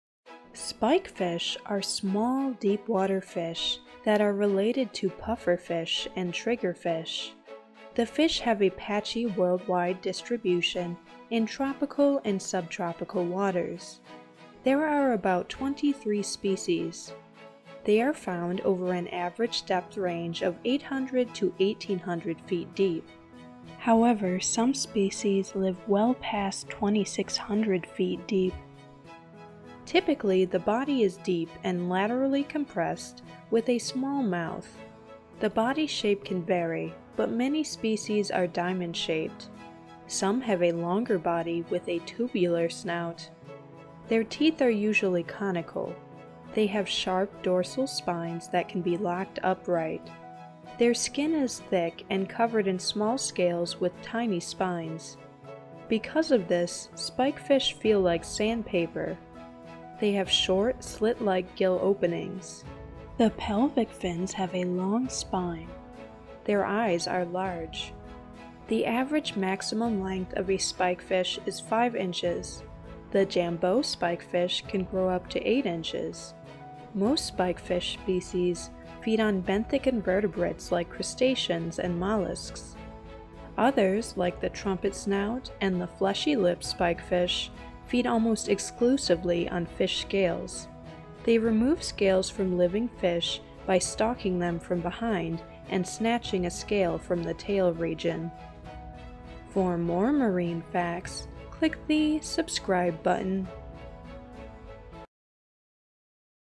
Facts: The Spikefish
Quick facts about this prickly deep sea fish! The spikefish (Triacanthodidae). Spikefish facts!

Snyder, David B., and Burgess, George H.. Marine Fishes of Florida. United States, Johns Hopkins University Press, 2016.

Priede, I. G.. Deep-Sea Fishes: Biology, Diversity, Ecology and Fisheries. United Kingdom, Cambridge University Press, 2017.

Robins, C. Richard, and Ray, Carleton. A Field Guide to Atlantic Coast Fishes: North America. United States, HMH Books, 2016.
Australian National Fish Collection, CSIRO.